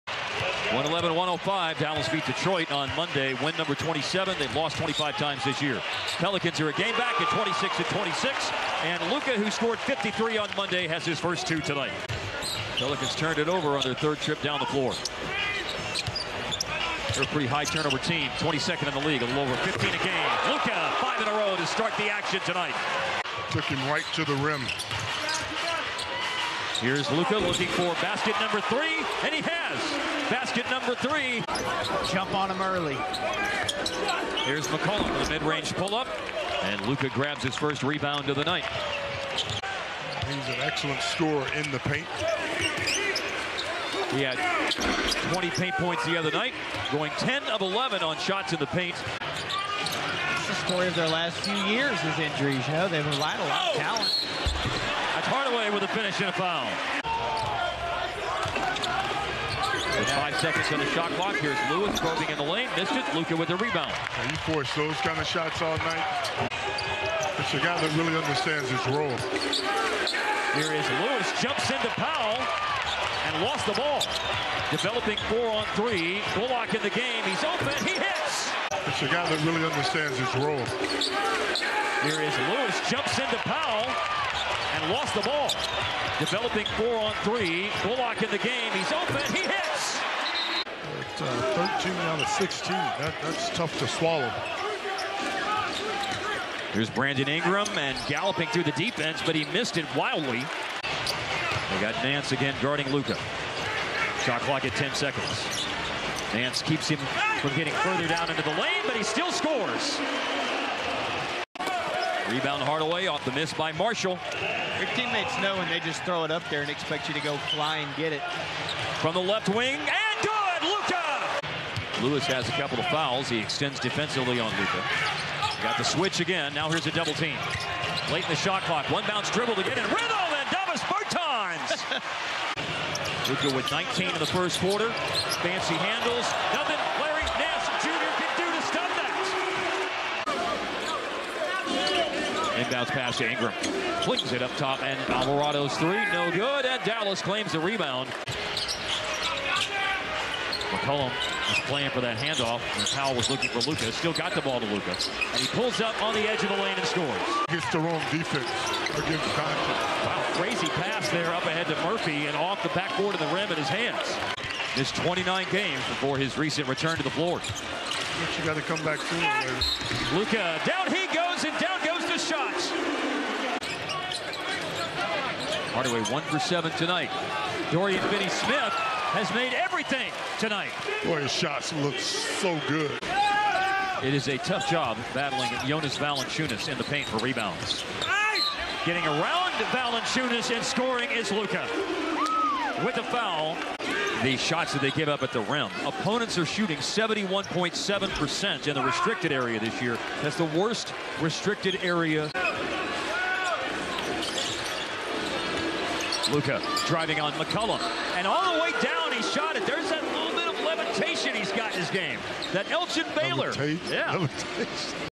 Luka Doncic vs New Orleans Pelicans | Feb 2, 2023
31 PTS
8 REB
4 AST

Luka Doncic vs New Orleans Pelicans
New Orleans Pelicans vs Dallas Mavericks